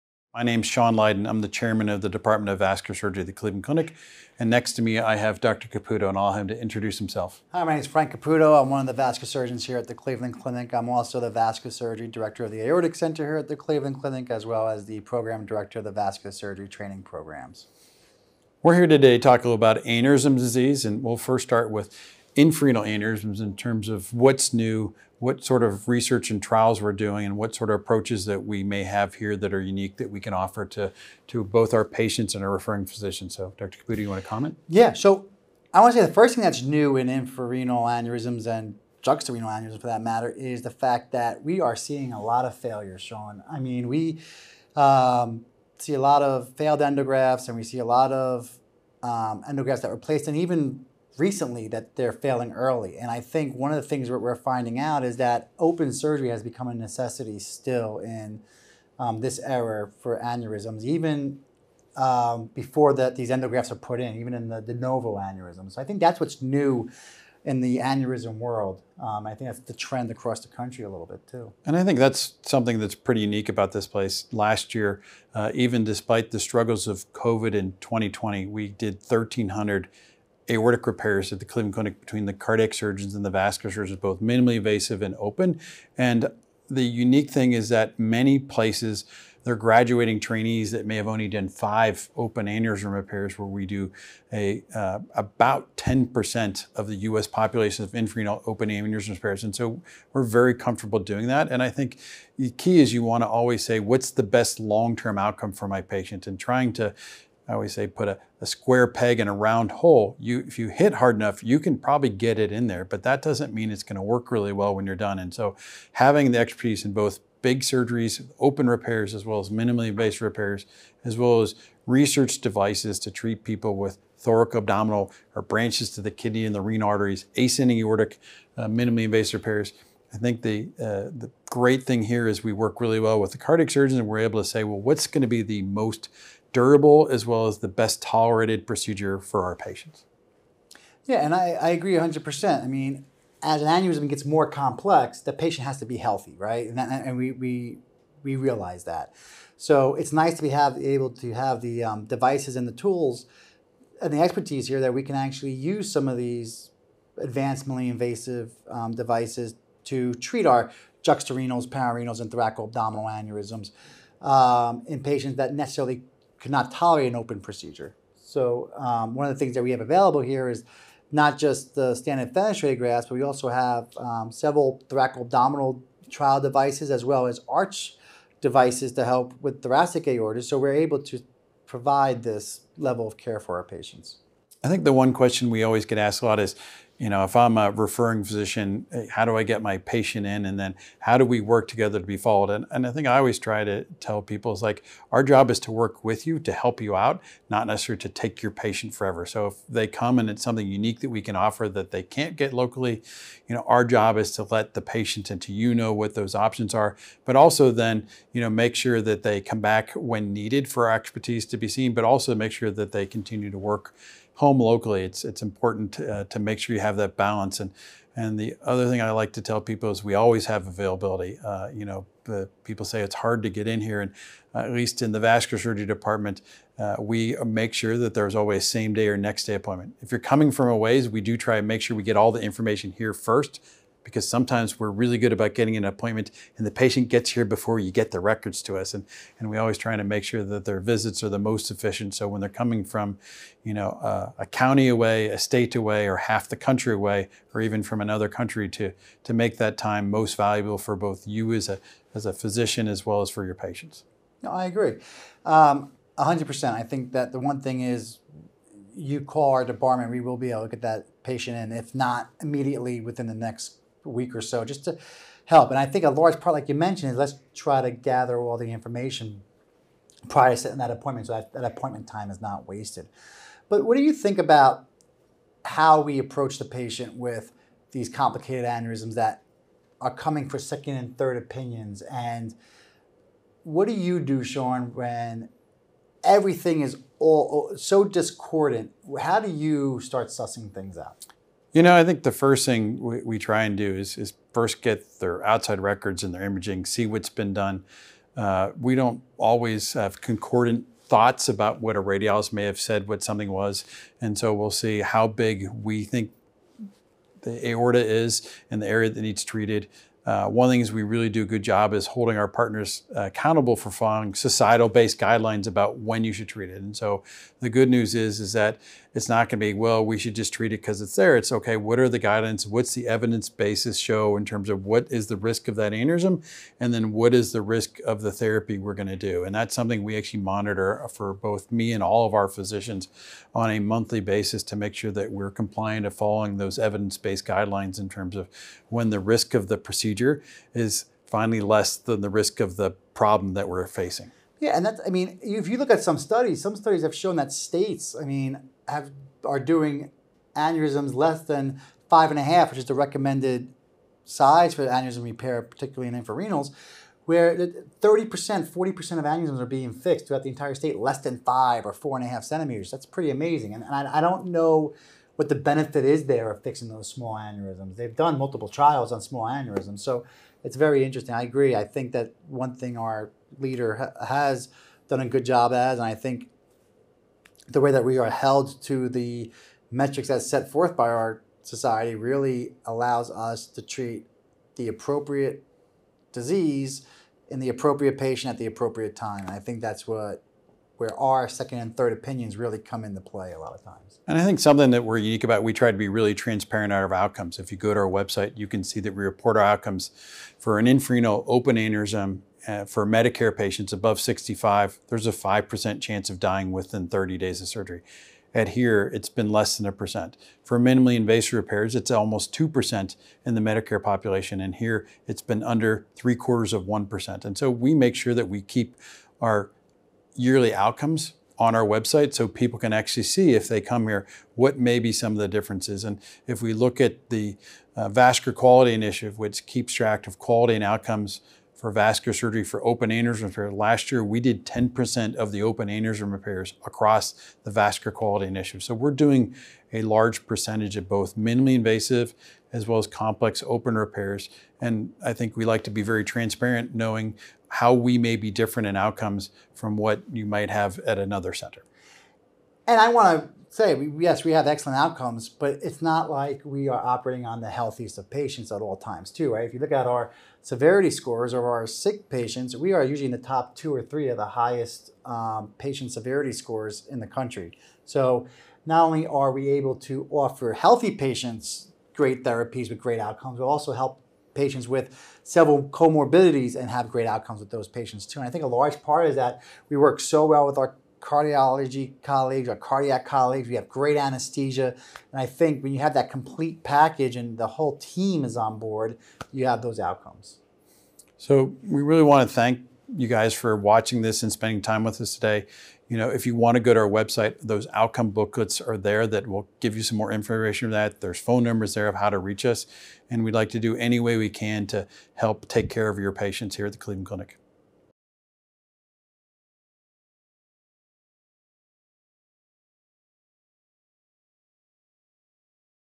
A Vascular Surgery Approach to Aortic Aneurysm Repair (For Healthcare Professionals)
The video is presented by Cleveland Clinic physicians for healthcare professionals.

There are many different treatment options for patients with aortic aneurysms. Dr. Sean Lyden and Dr. Francis Caputo discuss considerations and options to ensure patients are receiving appropriate treatment. The Vascular Surgery Department at Cleveland Clinic offers expertise in multiple treatment options and a focus on collaborative care.